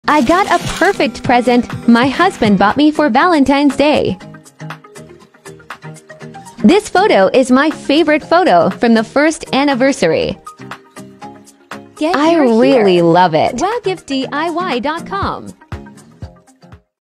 Custom Building Brick with Picture - Personalized Building Blocks Photo Valentines Day Gift For Her
Custom Building Brick with Picture - Personalized Building Blocks Photo Gift For Valentines Day
Create unique memories with Custom Building Blocks Photo! Use our customized building blocks to bring your images to life and capture precious moments. Our products are made from high-quality materials and designed to create stunning images. Watch this video to learn more about Custom Building Blocks Photo!